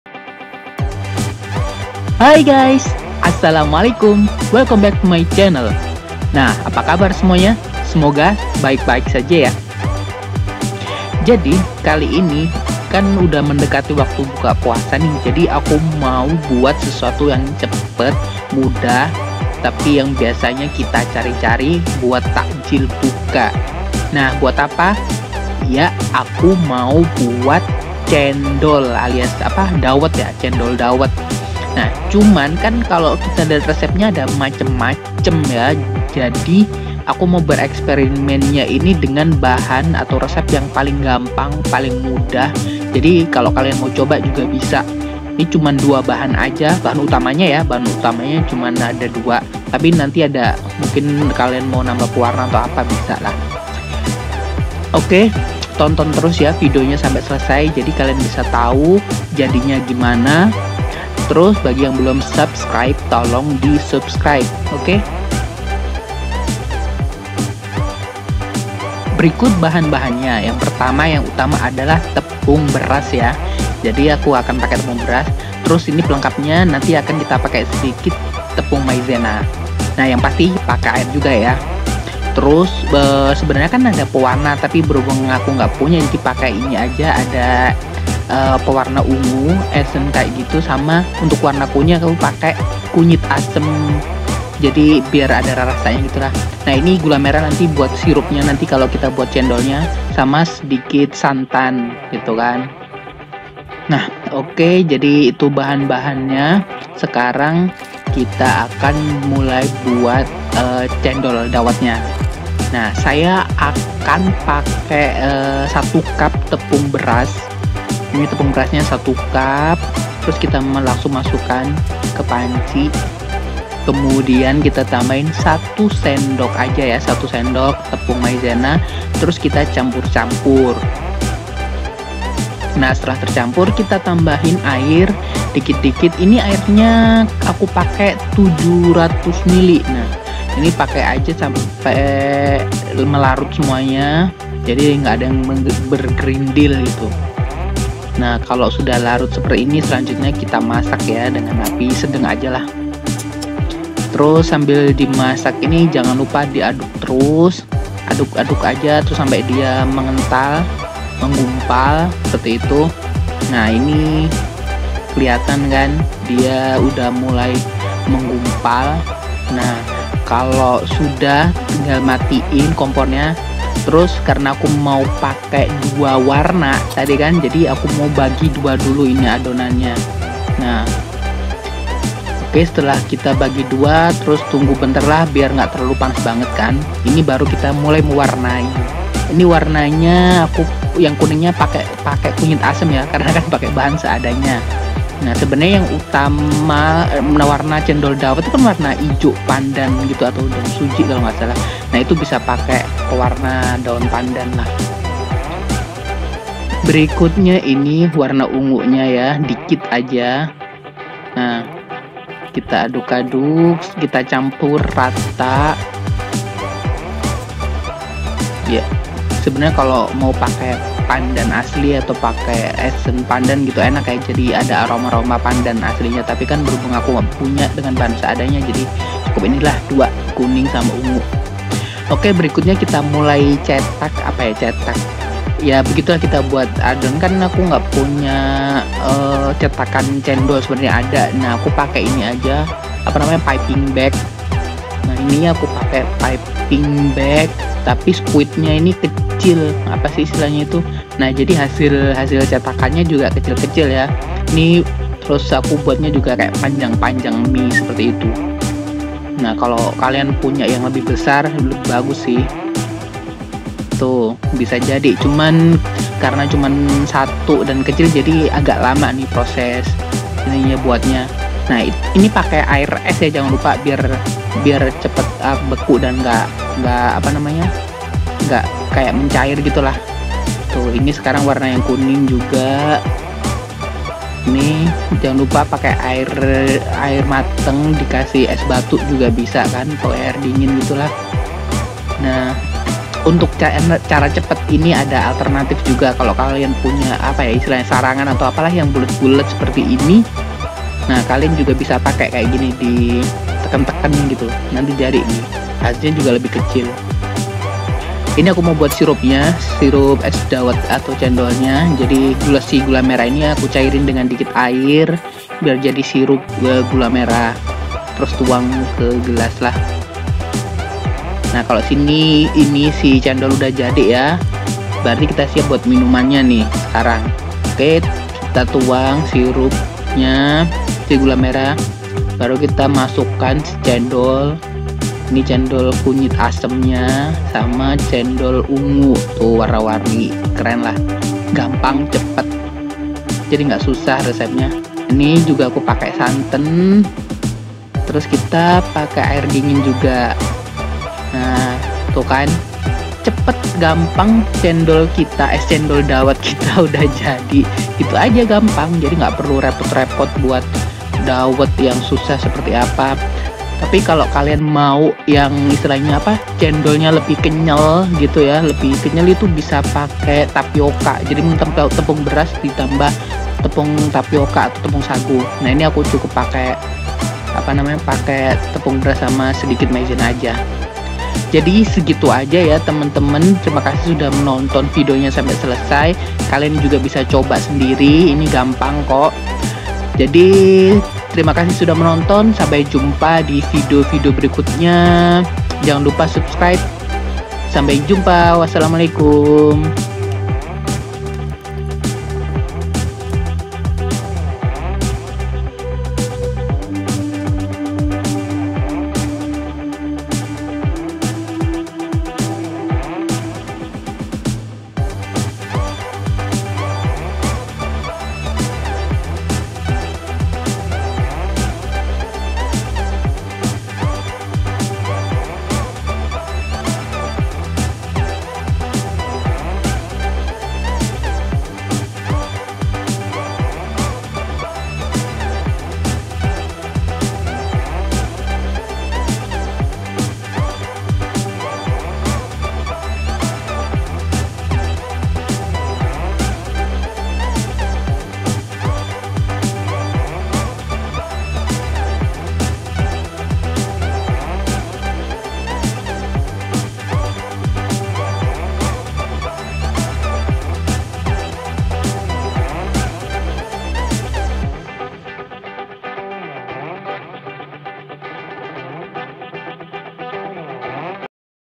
Cendol Dawet Kunyit Asem - Resep Mudah 3 Bahan Anti Gagal | Menu Ramadhan Lockdown
Assalamu'alaikum

Untuk menu takjil buka puasa hari ini, aku mau buat es cendol dawet. Pake resep yang gampang-gampang saja dan dengan bahan seadanya. Karena lagi corona jadi pewarnanya aku pakein kunyit asem instan juga biar lebih sehat. Cepat dan gk pake repot.
Selamat menjalankan ibadah puasa teman teman.

TERIMA KASIH